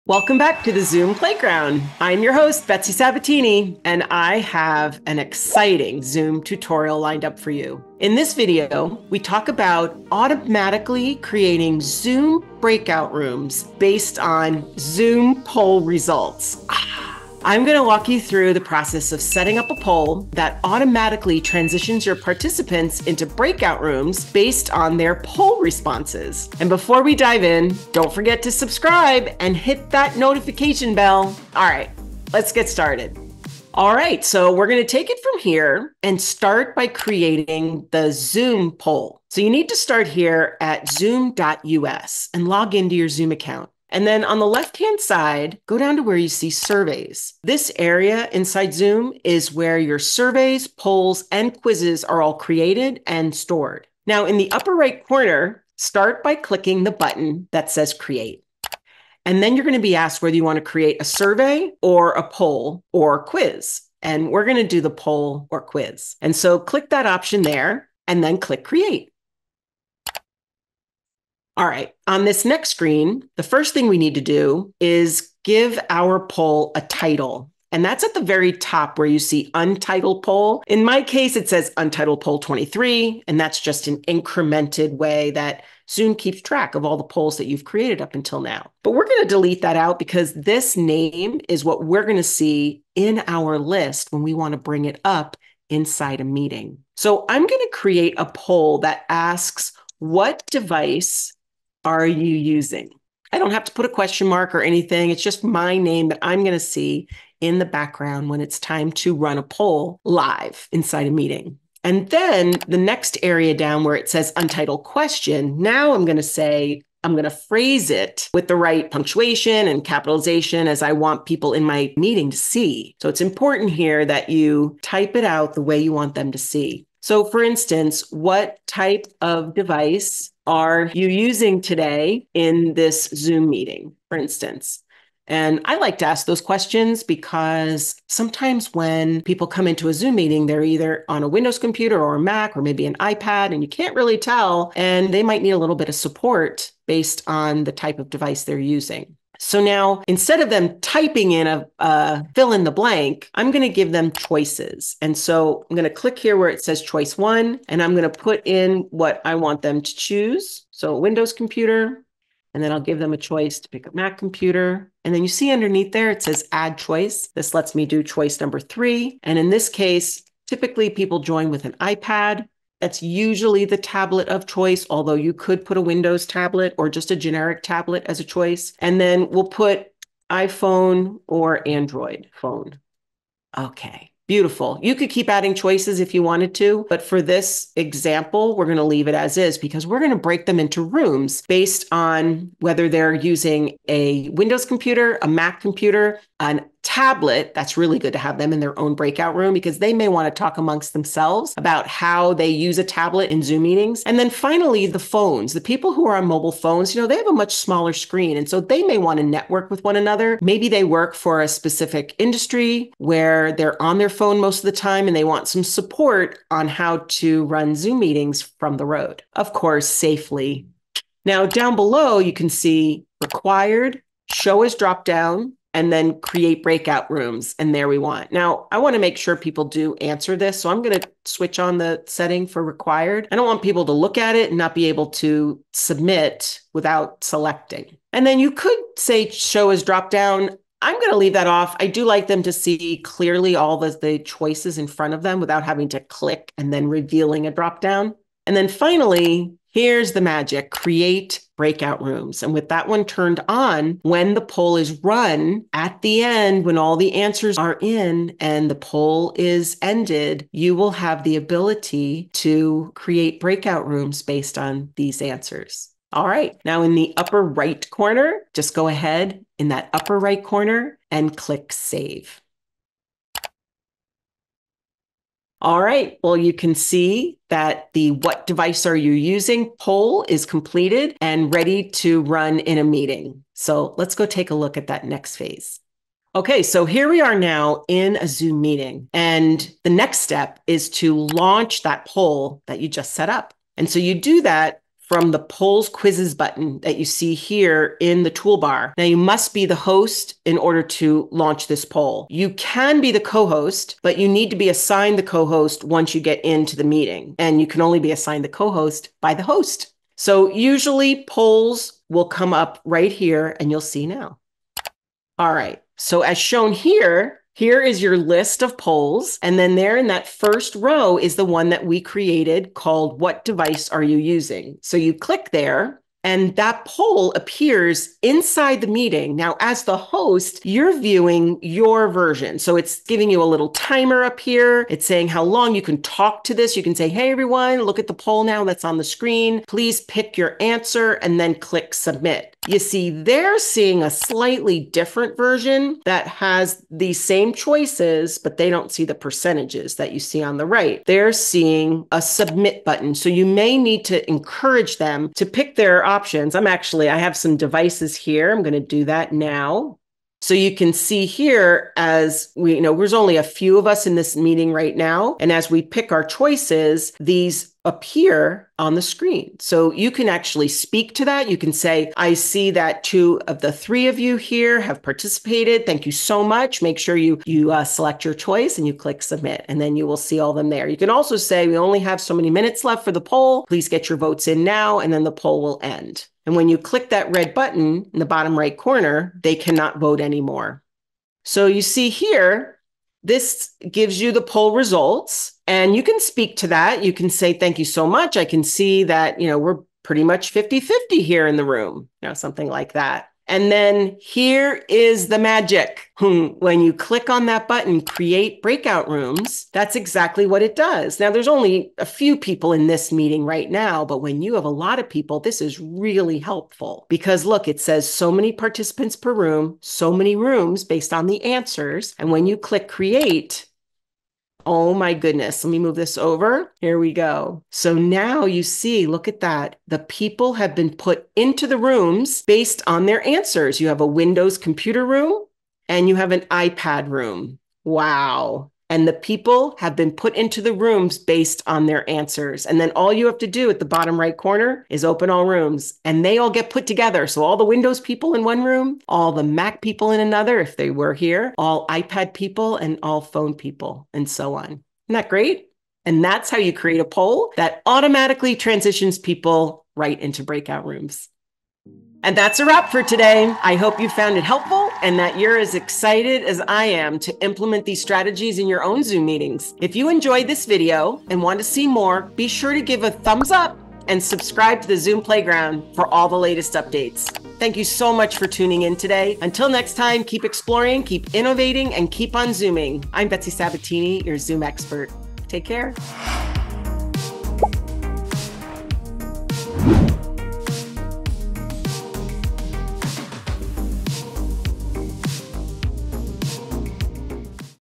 How to Create Zoom Breakout Rooms Based on Poll Results
Welcome back to The Zoom Playground! In this episode, we will dive into the exciting world of Zoom breakout rooms and automation.

In this tutorial, I will walk you through the step-by-step process of automatically creating Zoom breakout rooms based on poll results. Say goodbye to manual room assignments and hello to a more efficient, interactive meeting experience!

Timestamp:

0:00 - Introductions
0:33 - Creating a Zoom Poll
4:48 - Running a Zoom Poll
6:43 - Creating the Breakout Rooms
7:41 - Ending Spiels
8:05 - Closing Billboard